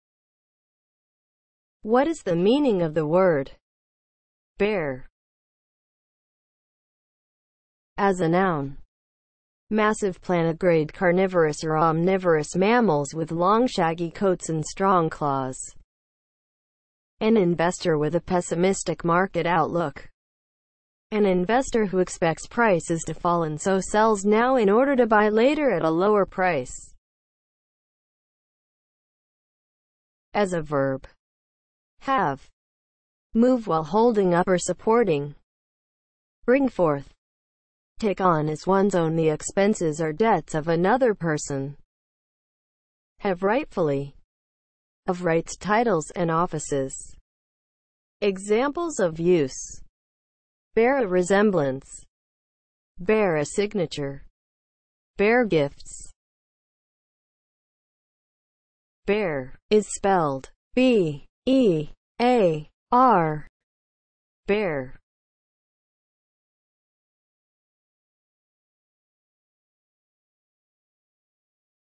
What is the meaning of the word BEAR?
Definition, examples of use and spelling of the word BEAR.

(00:00) Intro
(00:07) As a noun
(00:29) As a verb
(00:57) Spelling

As a noun (00:07):
• massive plantigrade carnivorous or omnivorous mammals with long shaggy coats and
  strong claws
• an investor with a pessimistic market outlook; an investor who expects prices to fall and so
  sells now in order to buy later at a lower price

As a verb (00:29):
• have
• move while holding up or supporting
• bring forth,
• take on as one's own the expenses or debts of another person
• have rightfully; of rights, titles, and offices

Examples:
• "bear a resemblance"
• "bear a signature"
• "Bear gifts"